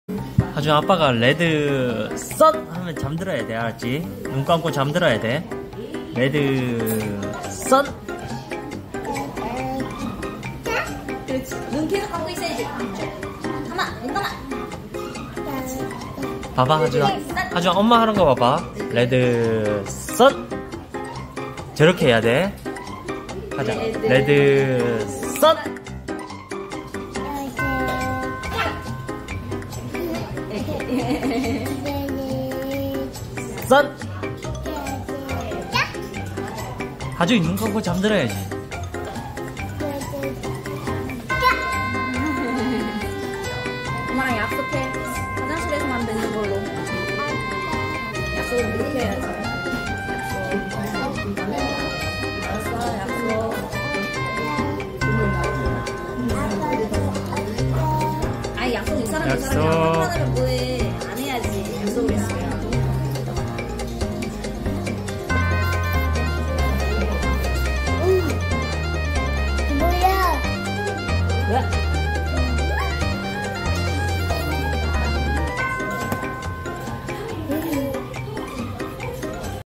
아빠가 레드썬! 하면 잠들어야돼 하준아~~ 과연 될까?
레드~~썬